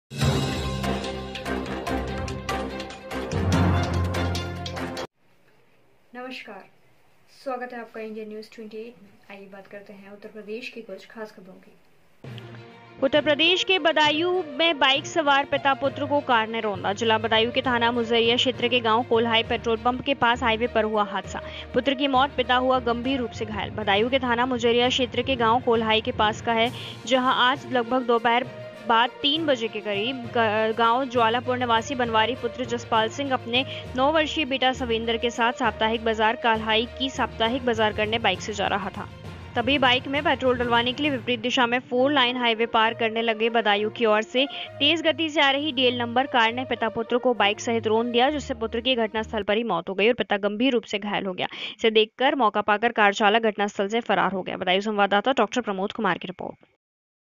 बदायूं बाइक सवार पिता पुत्र को कार ने रौंदा
Official